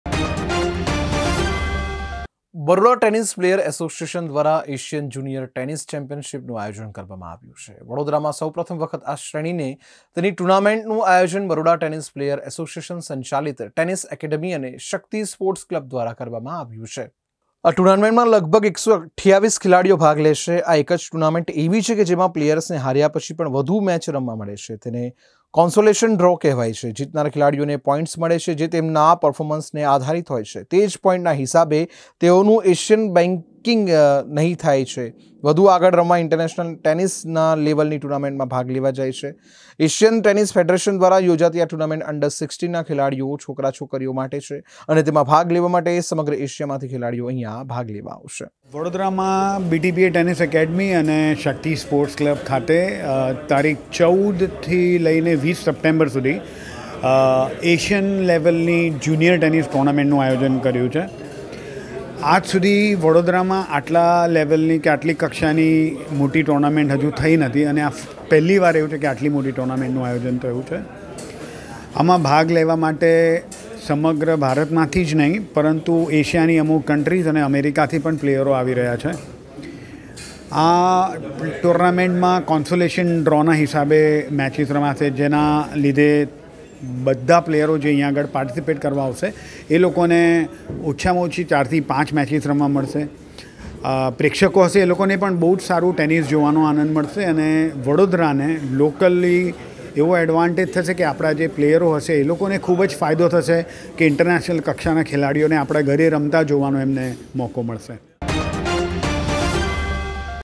એશિયન જુનિયર ટેનિસ ચેમ્પિયનશિપનું આયોજન | G One News
બરોડા ટેનિસ પ્લેયર એસોસિએશન દ્વારા એશિયન જુનિયર ટેનિસ ચેમ્પિયનશિપનું આયોજન..વડોદરામાં સૌપ્રથમ વખત આ શ્રેણીને તેની ટુર્નામેન્ટનું આયોજન બરોડા ટેનિસ પ્લેયર એસોસિએશન સંચાલિત ટેનિસ એકેડમી અને શક્તિ સ્પોર્ટ્સ ક્લબ દ્વારા કરવામાં આવ્યું છે..